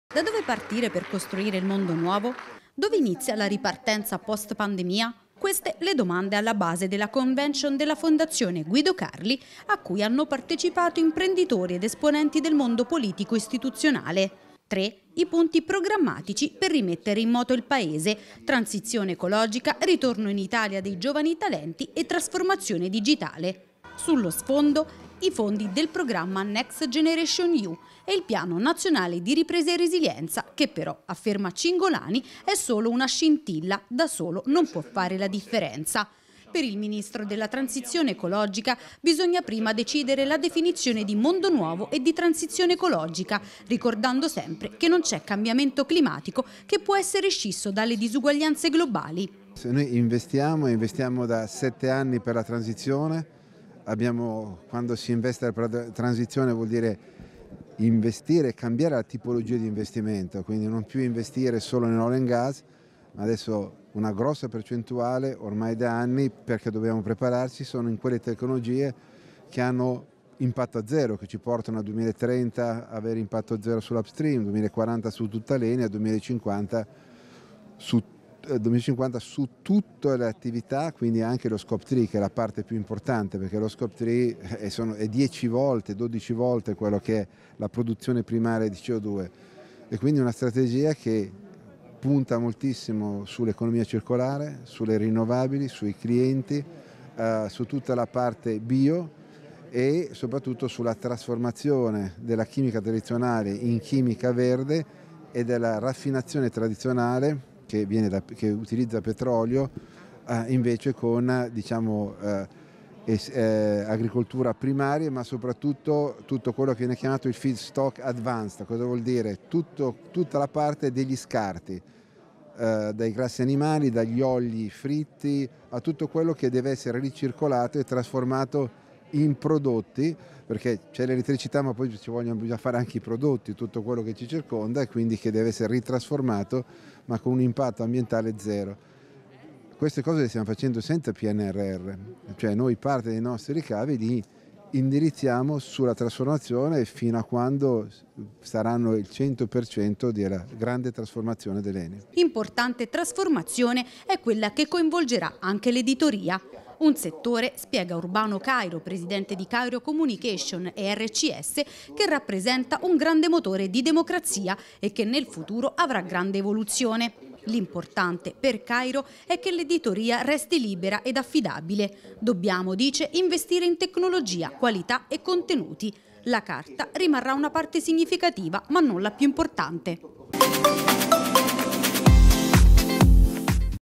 La transizione energetica ed "il mondo nuovo"
Da dove partire per costruire il mondo nuovo? Dove inizia la ripartenza post pandemia?
Queste le domande alla base della Convention della Fondazione Guido Carli
a cui hanno partecipato imprenditori ed esponenti del mondo politico istituzionale.
Tre i punti programmatici per rimettere in moto il Paese: Transizione ecologica, ritorno in Italia dei giovani talenti e trasformazione digitale.